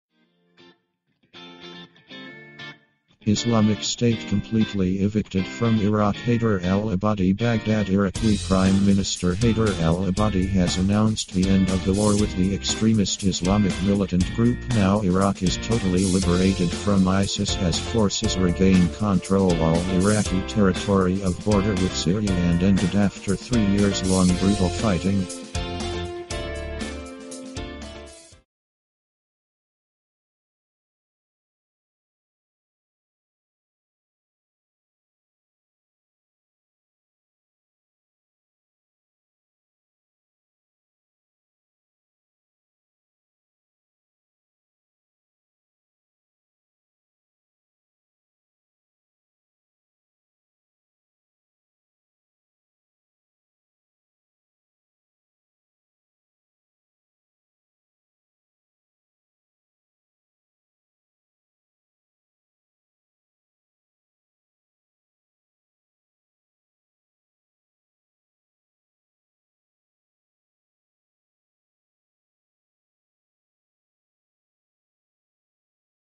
ISLAMIC STATE COMPLETELY EVICTED FROM IRAQ HAIDER AL ABADI
BAGHDAD IRAQUI P M ANNOUNCED THE END OF THE WAR WITH THE EXTREMIST ISLAMIC MILITANT GROUP NOW IRAQ IS TOTALLY LIBERATED FROM ISIS  & OUR FORCE REGAIN CONTROL ALL TERITORY BORDER WITH SIRYA & ENDED AFTER THREE YEARS LONG BRUTAL FIGHTING